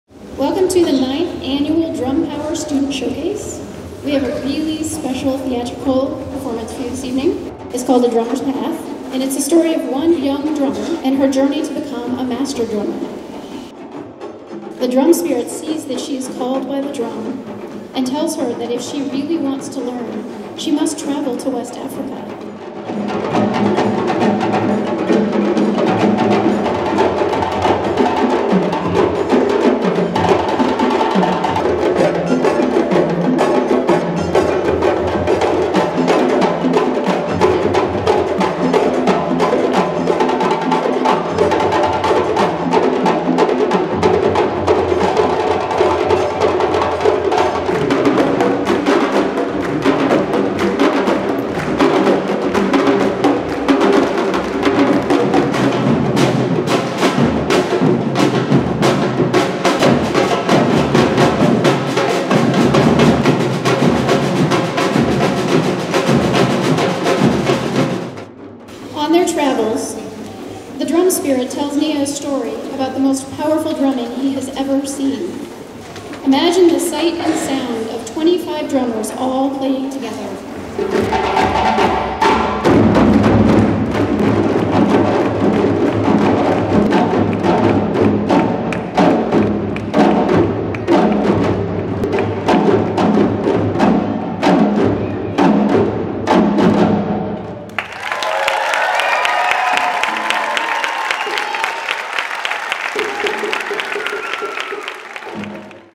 Drum Power Africa Night 2019 Promo
Annual Drum Power student recital featuring a play set in Old Mali as one young drummer becomes a master drummer--A Drummer's Path.